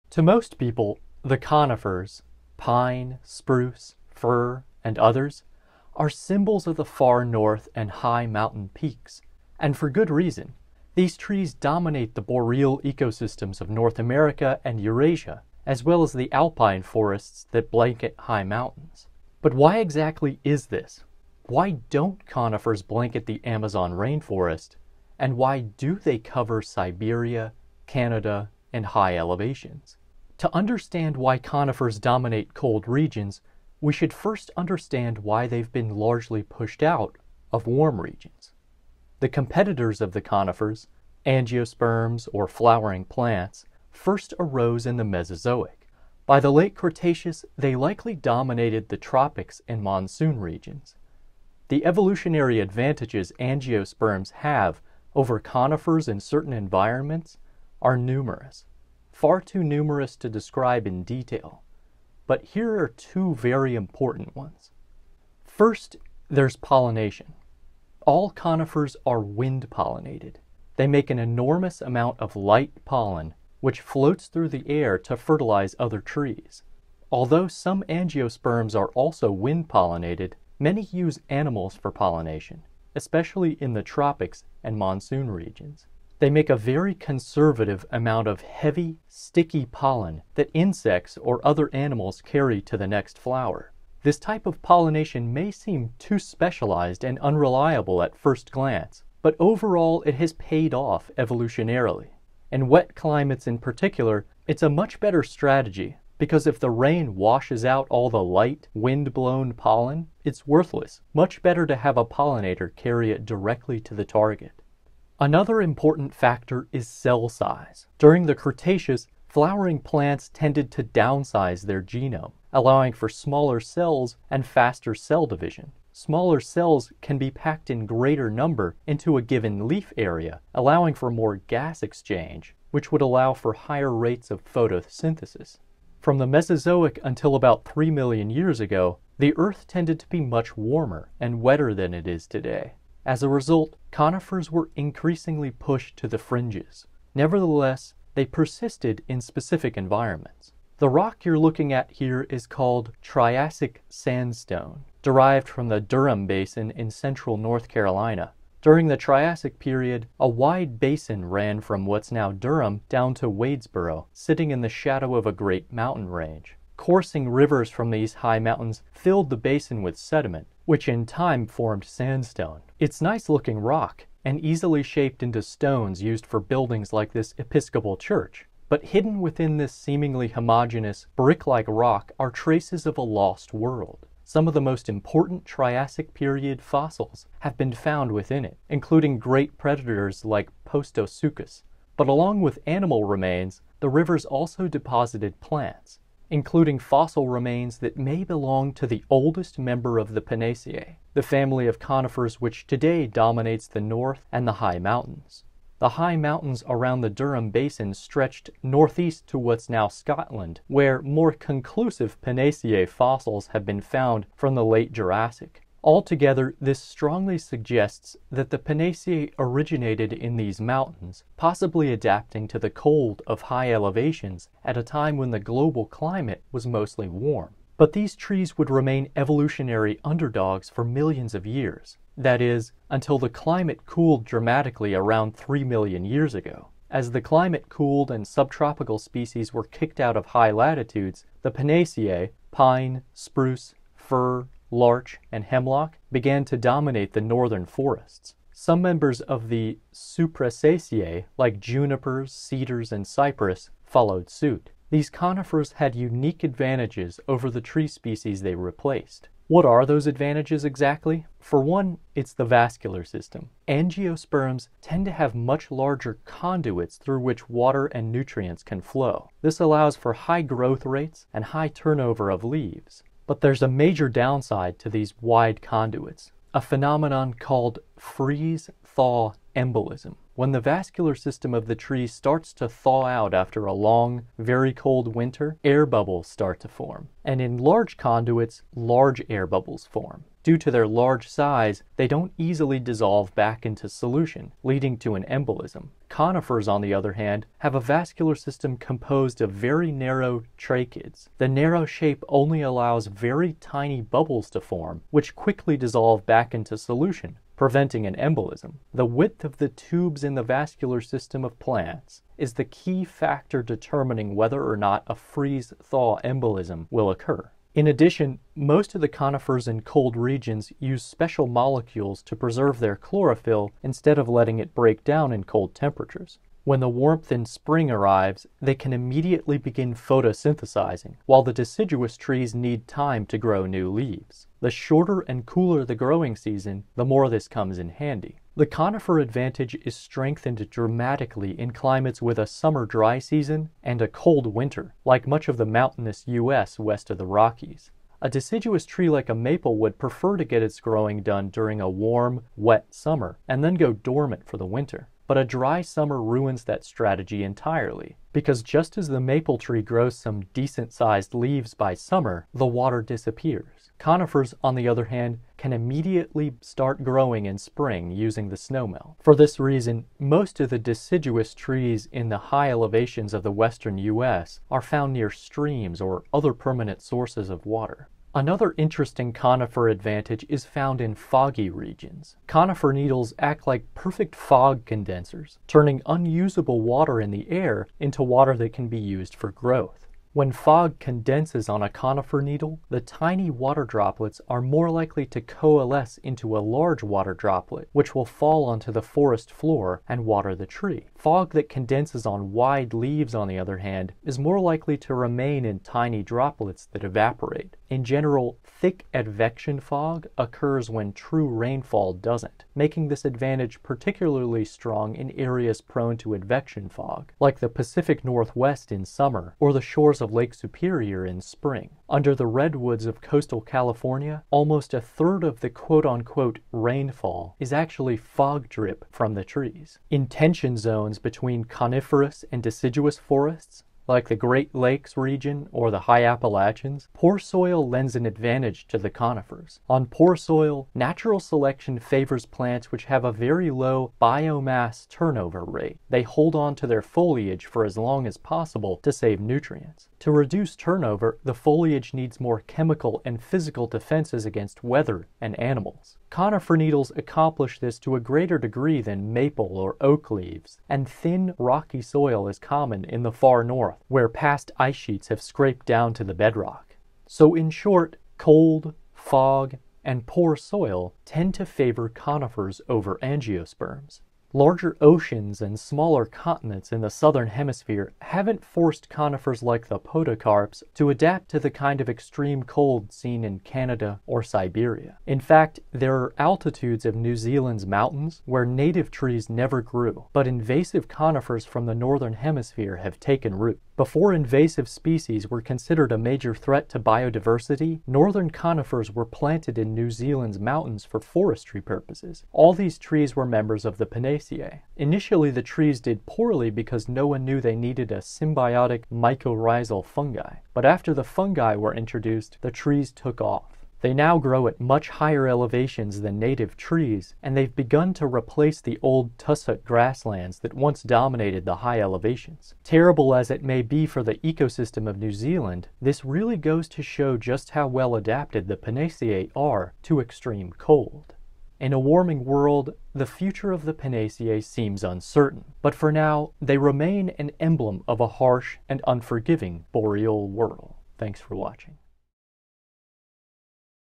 Why Do Conifers Rule the North?
This time of year, many people are bringing conifers into their homes to decorate. For many of us, they symbolize the winter season. But it’s easy to take these trees for granted—where do these pine, spruce, fir, and their relatives come from? Why do they dominate the frozen fringes of the far north in Siberia and Canada, as well as high mountains? How do they endure a winter colder than any other tree can?

Video sources:
Daniel Davis (self)

Image sources:
Wikipedia commons

Information Sources:

Fog Collection on Plant Surfaces and Biomimetic Applications. Mathematics and Natural Sciences Department, University of Bonn. MD Abul Kalam Azad, January 2016.

T. E. Dawson (September 1998). "Fog in the California redwood forest: ecosystem inputs and use by plants". Oecologia. 117 (4): 476–485.

Stewart T. Schultz (1990).  The Northwest Coast: A Natural History.  Portland, Oregon, Timber Press.

Waring, RH, and JF Franklin. (1979).  Evergreen coniferous forests of the Pacific Northwest.  Science 204: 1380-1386.

Waring, RH, (1982) Land of the giant conifers.  Natural History 91(10):54-63.

Analysis of Freeze-Thaw Embolism in Conifers. The Interaction between Cavitation Pressure and Tracheid Size
Jarmila Pittermann and John S. Sperry

Elegance versus Speed: Examining the Competition between Conifer and Angiosperm Trees
Timothy J. Brodribb, Jarmila Pittermann, and David A. Coomes

“Although unambiguous Pinaceae are not known before the Late Jurassic, fossils of putative stem members of the family appear during the Triassic and possibly as early as the Late Permian”
Ovulate Cones of Schizolepidopsis ediae sp. nov. Provide Insights into the Evolution of Pinaceae
Kelly K. S. Matsunaga, Patrick S. Herendeen, Fabiany Herrera, Niiden Ichinnorov, Peter R. Crane, and Gongle Shi

Fertile Coniferophyte Remains from the Late Triassic Deep River Basin, North Carolina. T. Delevoryas and R. C. Hope. American Journal of Botany

Early evolution in the Pinaceae. Charles N. Miller Jr. Review of Palaeobotany and Palynology.

Pollination Biology, Leslie Real, December 2012.

Simonin KA, Roddy AB (January 2018). "Genome downsizing, physiological novelty, and the global dominance of flowering plants". PLOS Biology